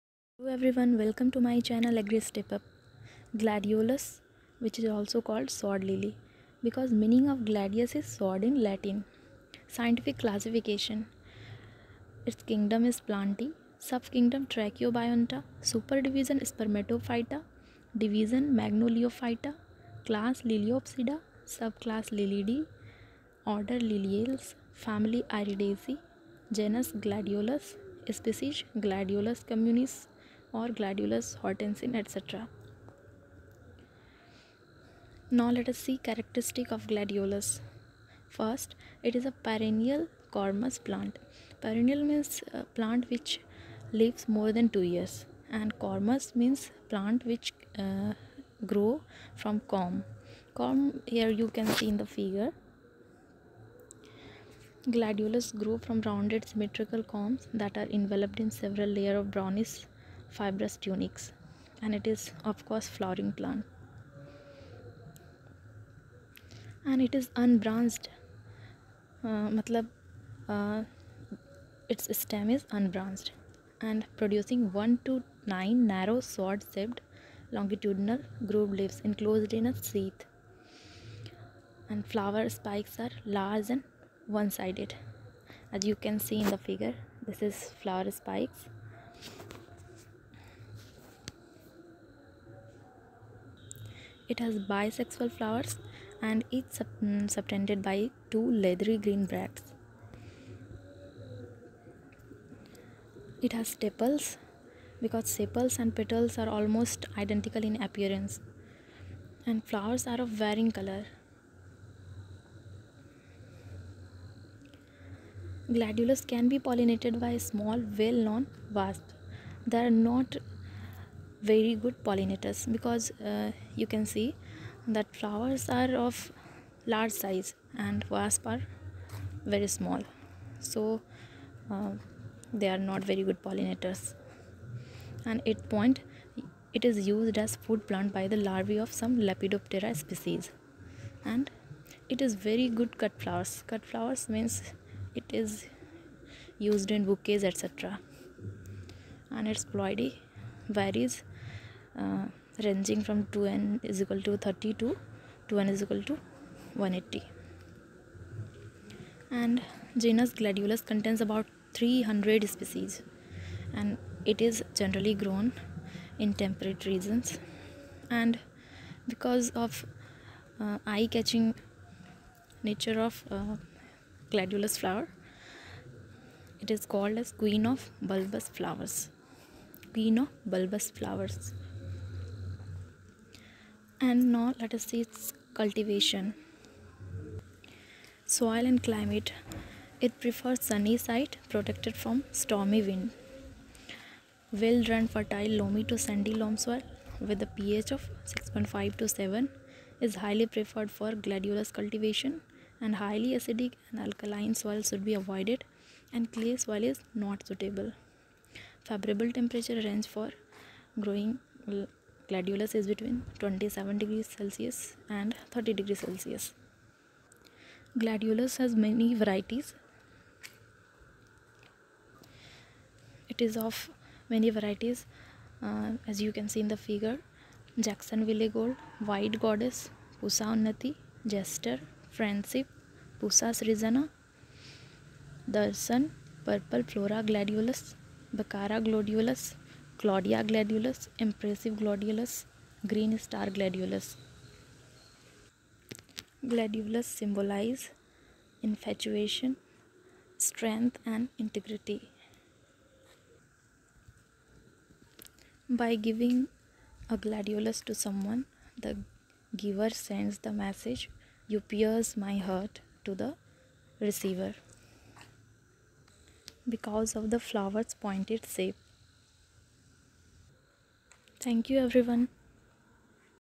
Gladiolus (Sword Lily)
Gladiolus is a perennial, cormous, ornamental cut flowers.
For their different varieties, soil & climatic conditions and many characteristics, please watch the video till the last.